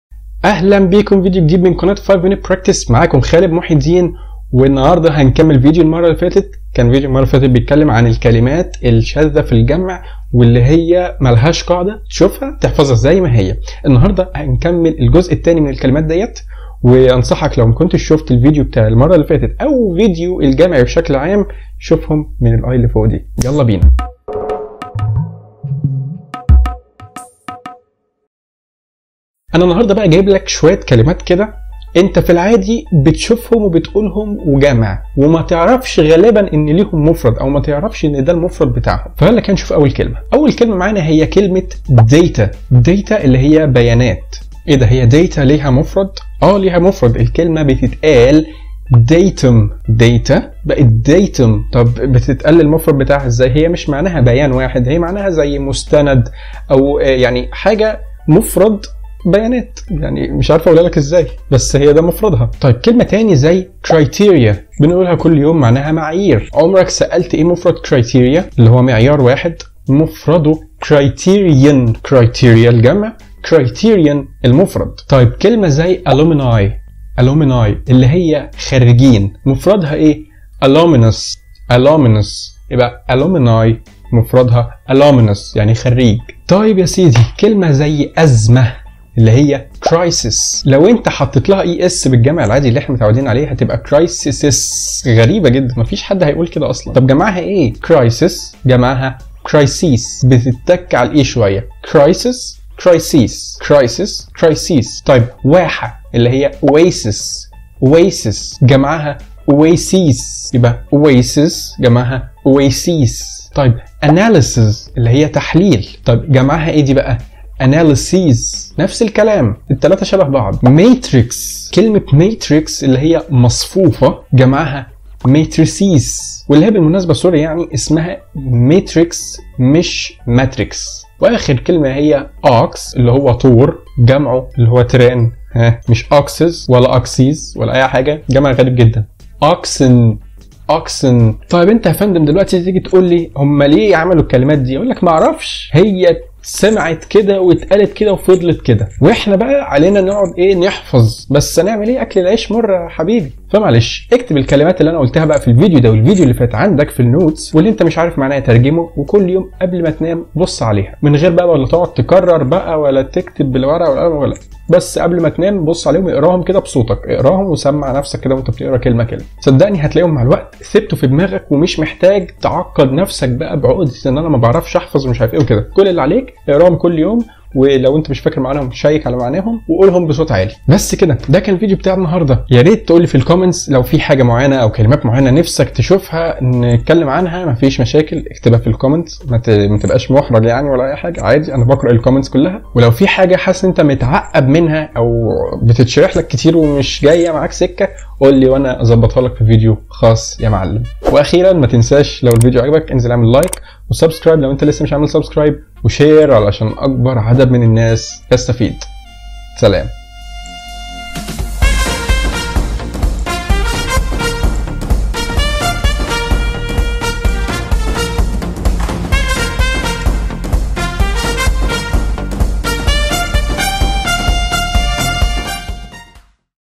جمع الكلمات الشاذه فى الانجليزى | الجزء الثانى
جمع الكلمات الشاذه فى الانجليزى والصور المختلفة لها  والكلمات الاكثر شيوعا
ــــــــــــــــــــــــــــــــــــــــــــــــــــــــــــــــــــــــــــــــ
ــــــــــــــــــــــــــــــــــــــــــــــــــــــــــــــــــــــــــــــــ
Music